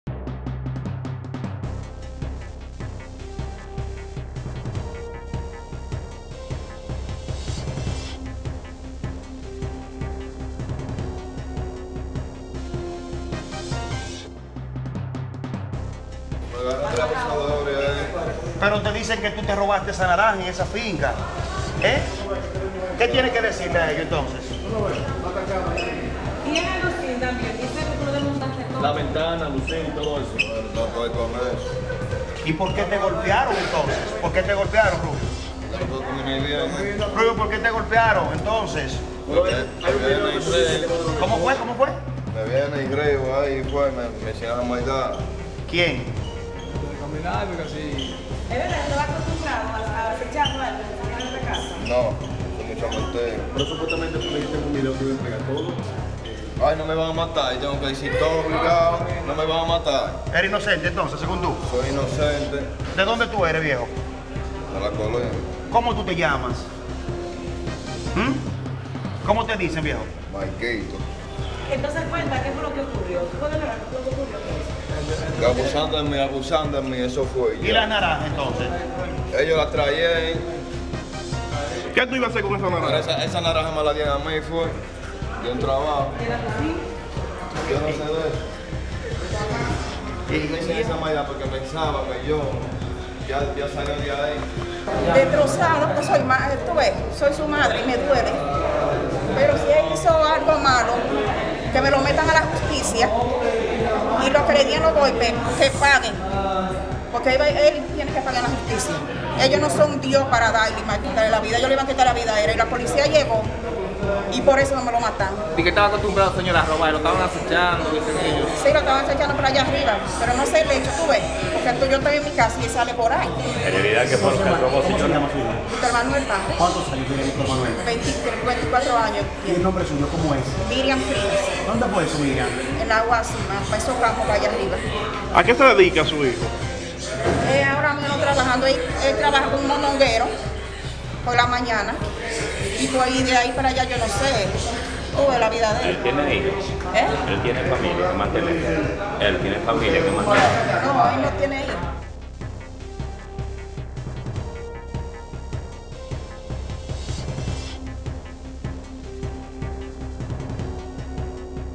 Apresan y golpean joven tras presuntamente sorprenderlo robando dentro de finca en SFM
San Francisco De Macorís.- Los residentes en Las Guázumas propinaron una golpiza a un joven luego de presuntamente sorprenderlo robando en el interior de una finca.

Se trata de Victor Manuel Parra, de 25 años de edad, quien fue trasladado al Comando Regional Nordeste de la Policía, quien es señalado por robar frutas, entre otros asuntos, dentro de la propiedad.

Sin embargo, Parra negó las acusaciones y dijo que confesó para evitar que lo matasen.

De su lado, la madre del joven pidió justicia por su hijo ya que no se merecía que lo golpearan, pero admitió que si ha cometido algún crimen debe ser puesto en prisión.